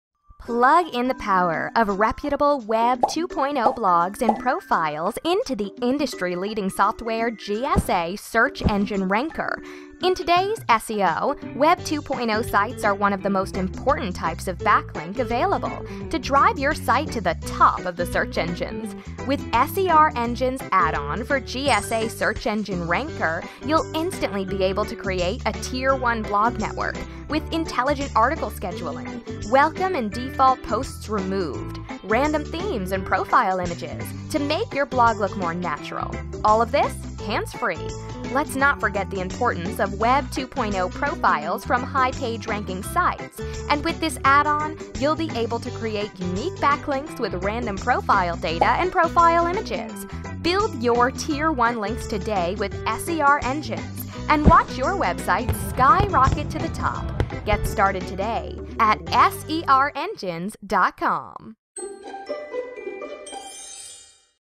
SEREngines.com GSA Search Enginer Ranker Addon
SEREngines is a Web 2.0 blog and profile addon for the industry leading SEO software GSA Search Engine Ranker.